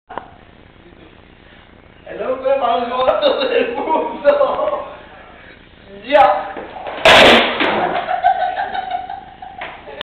Abel el Hombre mas grande del mundo
El hombre mas grande del mundo rompe una silla xD